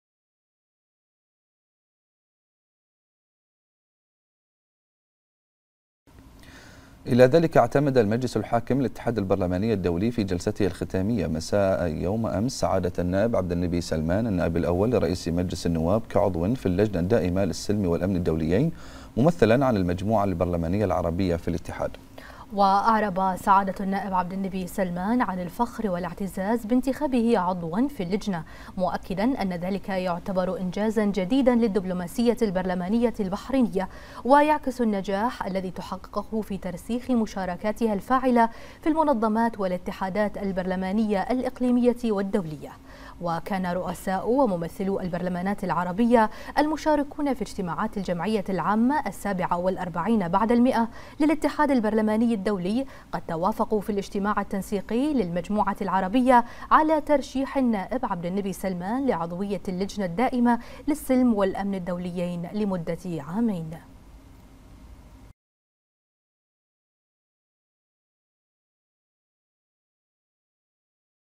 اعتماد النائب عبدالنبي سلمان عضوًا في اللجنة الدائمة للسلم والأمن الدوليين بالبرلماني الدولي
مجلس النواب  - مركز الأخبار - تلفزيون البحرين